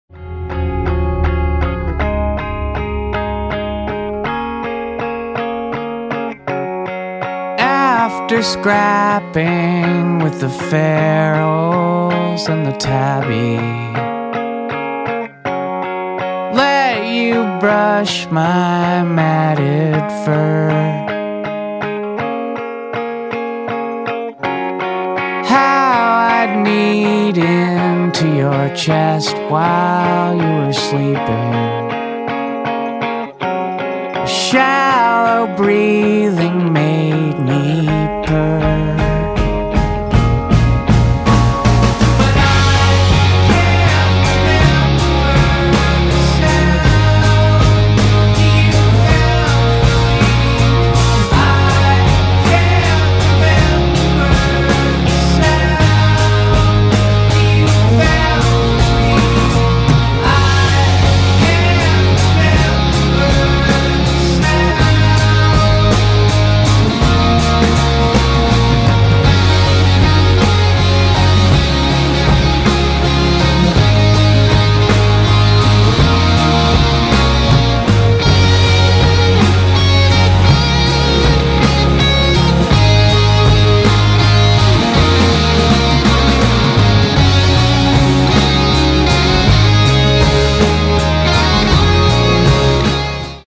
Cat draws Taj Mahal on Etch-a-Sketch
Groundbreaking! Cat draws Taj Mahal on etch-a-sketch!